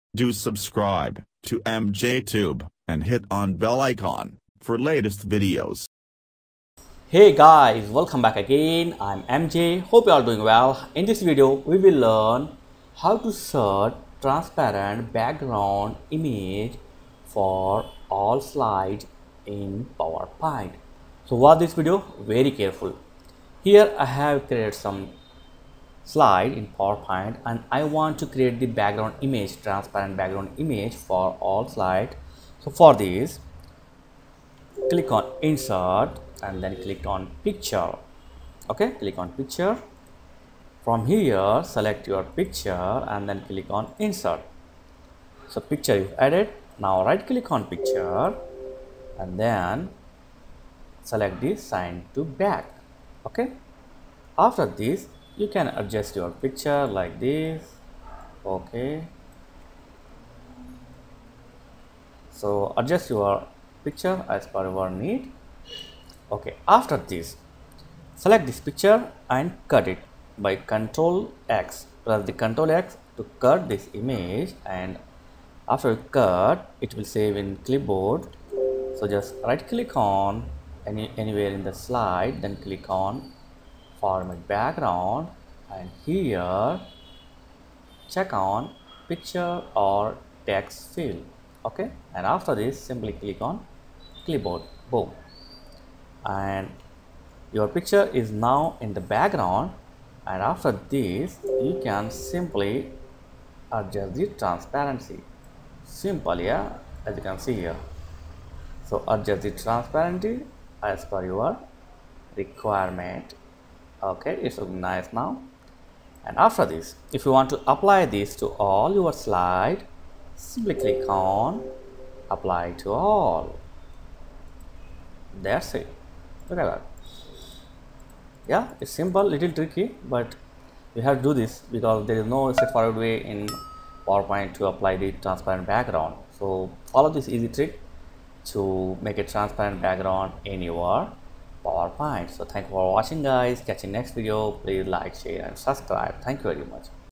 PowerPoint: Set Transparent Background Picture to All Slides
How to set Transparent Background Picture to All PowerPoint Slides